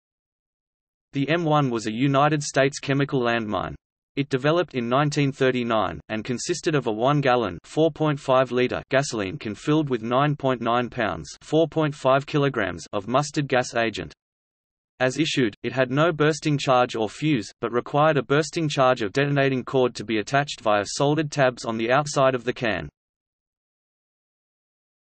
M1 chemical mine | Wikipedia audio article
- Socrates

SUMMARY
The M1 was a United States chemical landmine. It developed in 1939, and consisted of a one gallon (4.5 liter) gasoline can filled with 9.9 lbs (4.5 kg) of mustard gas agent. As issued, it had no bursting charge or fuze, but required a bursting charge of detonating cord to be attached via soldered tabs on the outside of the can.